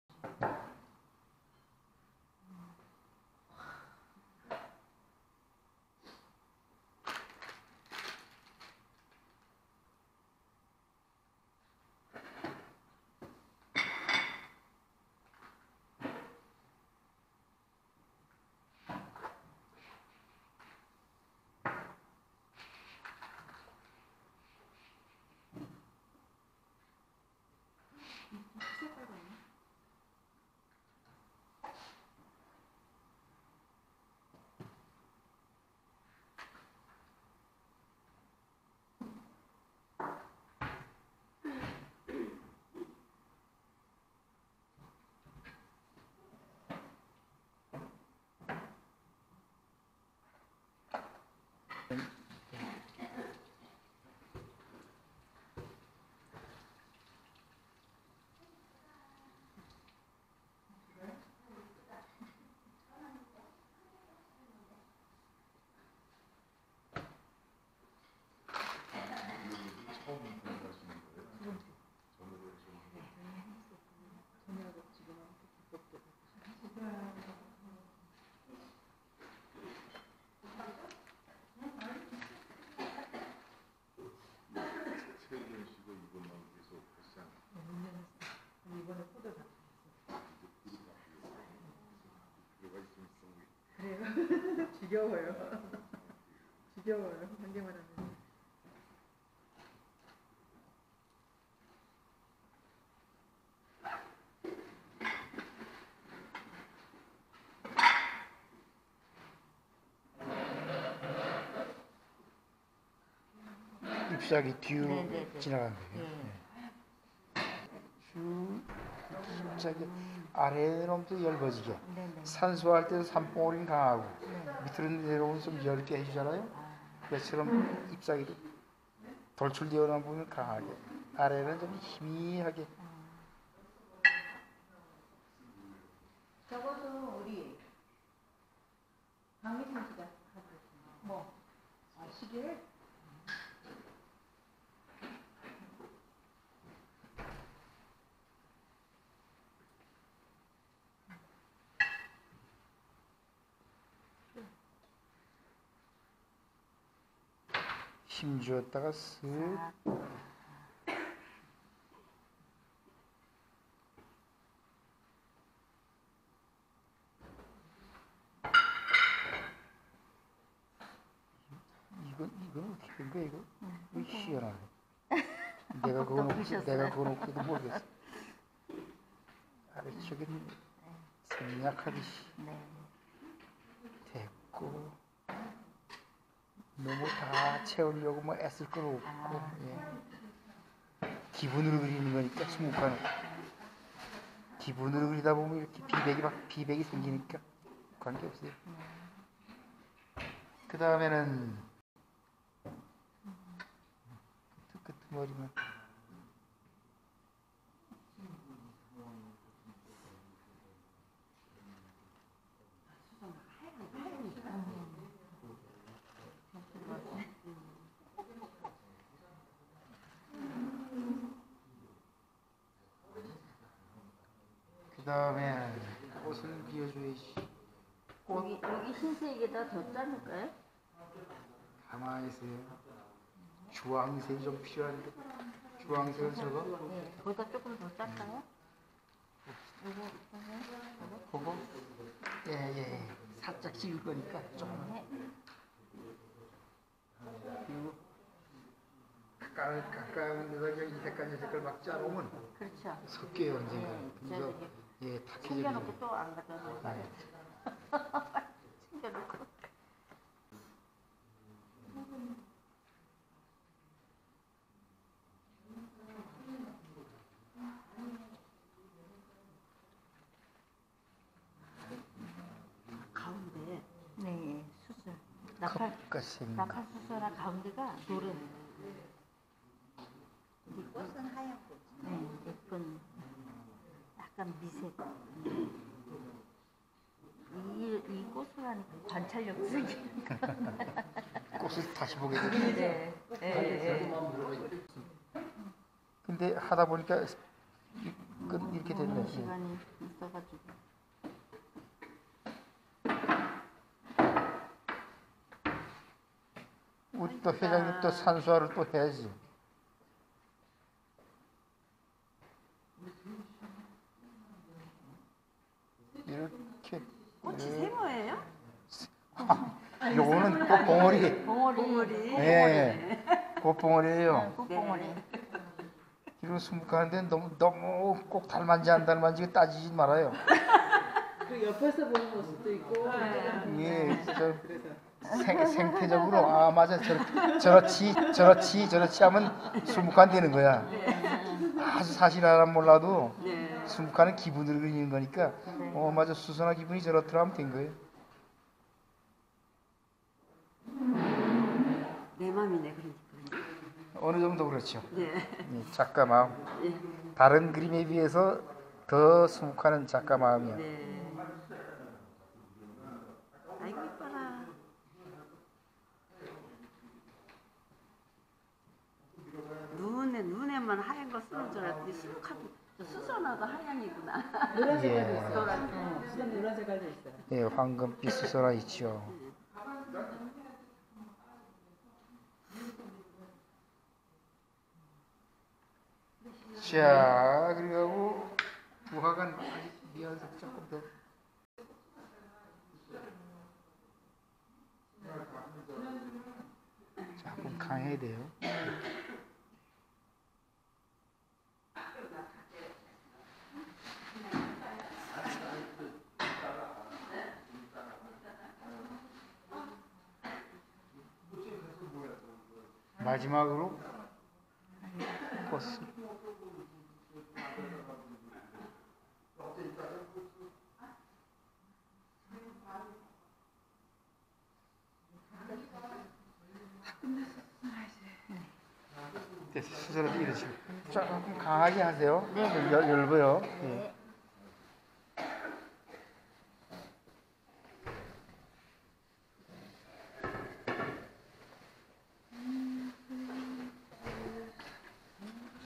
하늘밭 수묵화 교실-수선화 그리기 Ink painting Daffodil
수묵화, 한국화, 동양화, 사군자, 화조화, 산수화, 풍경화를 그리는 요령을 시연해 보이고 있습니다.